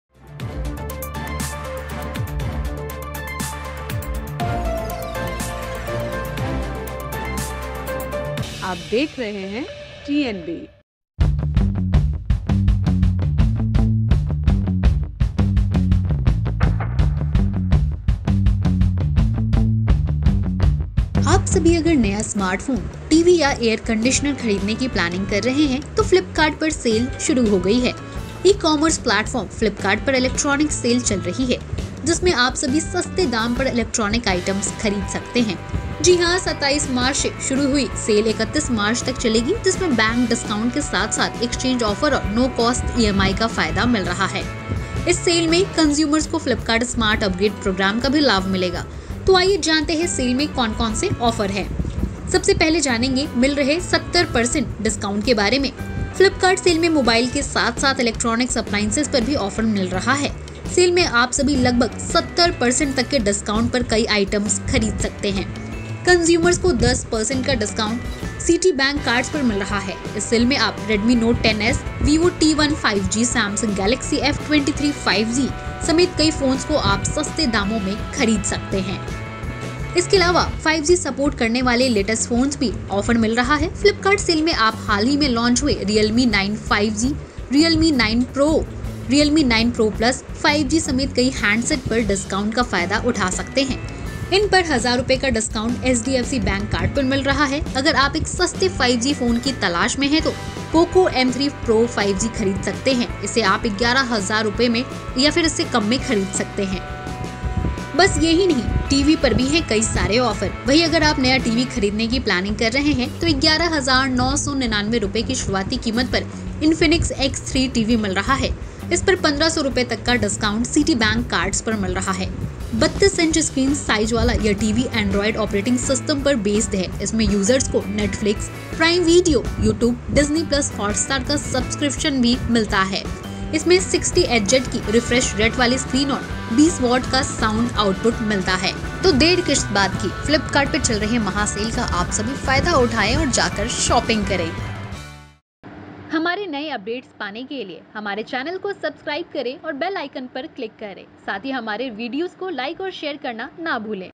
Flipkart Electronics Sale : Flipkart पर 70% Discount में Smartphone,TV और Electronics Appliances
Flipkart Electronics Sale: Avail discounts of up to 70% on Smartphones, TV and other Electronic Items.

For all the shopaholics out there, Flipkart is offering massive discounts on all Electronic Items. So if in case you have been eying on buying some Electronics Stuffs, this is a great time to splurge. From the 27th to the 31st of March, Flipkart is hosting a Massive Electronic Sale. Apart from the bank discounts, various exchange offers and No Cost EMIs options will also be available to the customers. The consumers will also be benefitted by the Flipkart Smart Upgrade Programme.